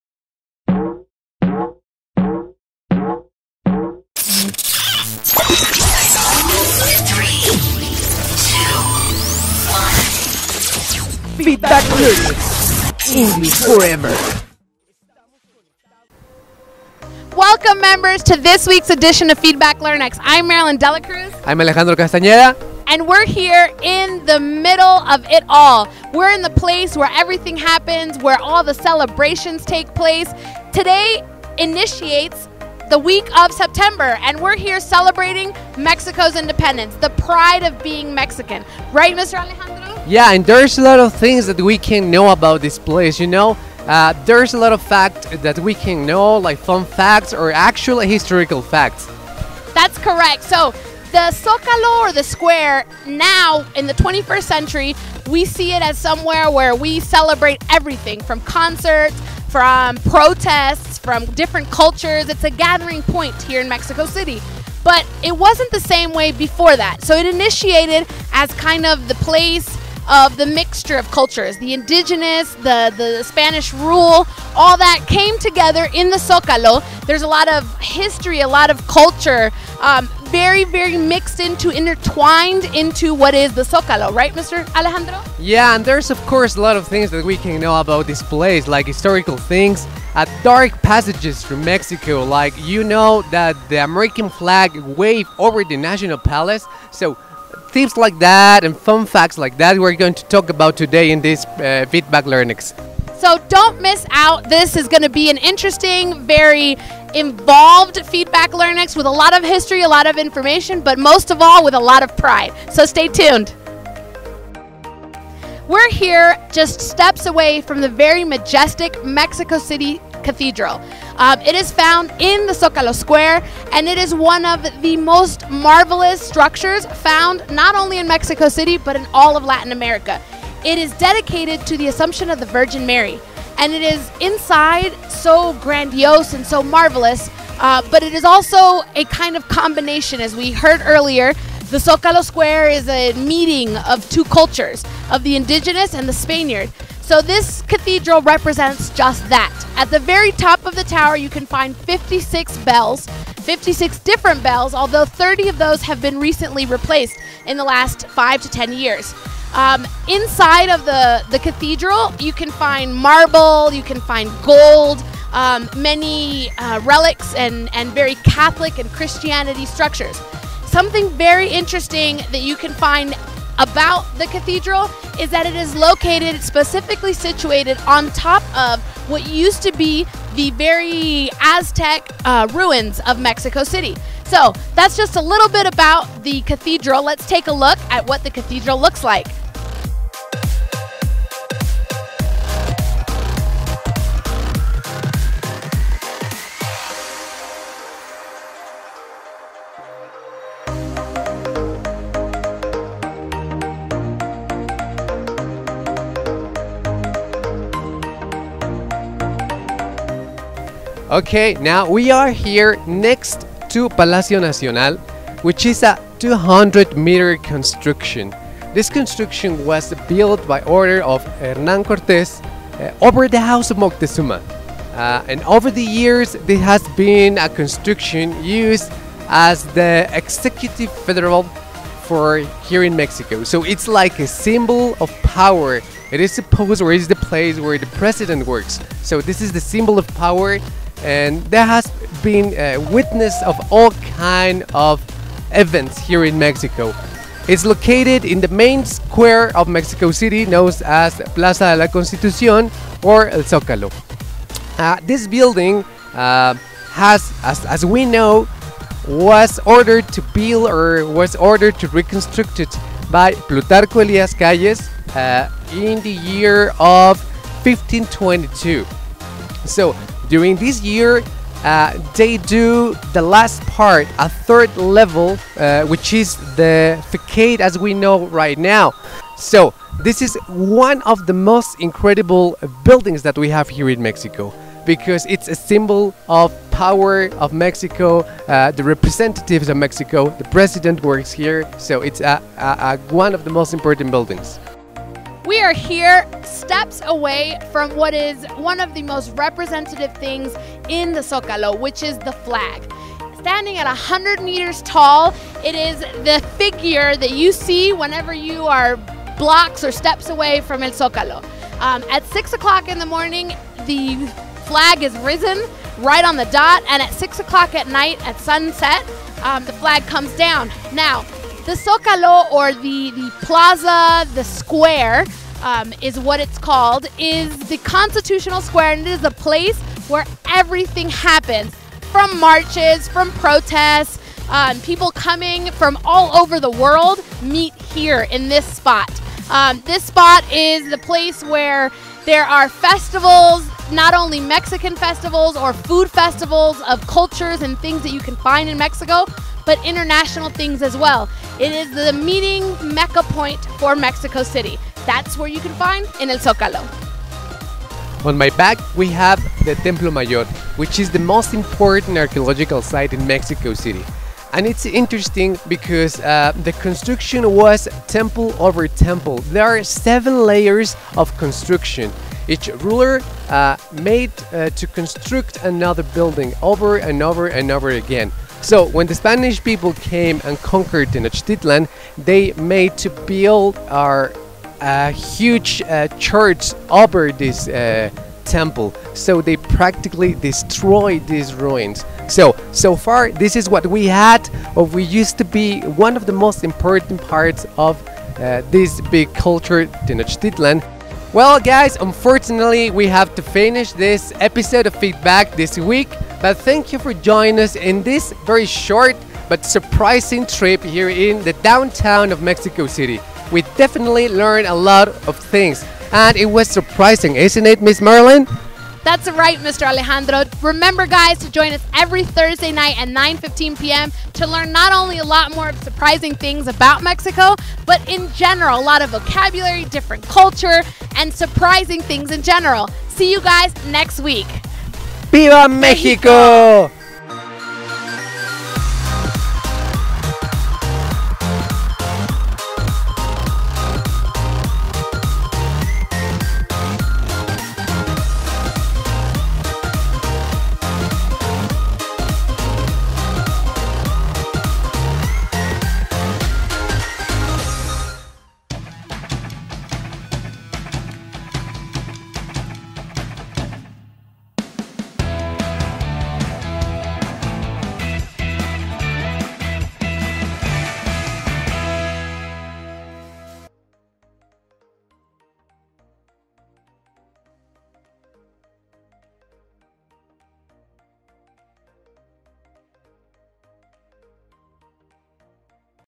The Pride of Being Mexican
How do you celebrate the Mexican Independence? On this episode we talk about the traditions and customs we have for this very special night.

¿Cómo celebras la Independencia de México? En este episodio hablamos sobre las tradiciones y costumbres que tenemos para esta noche especial.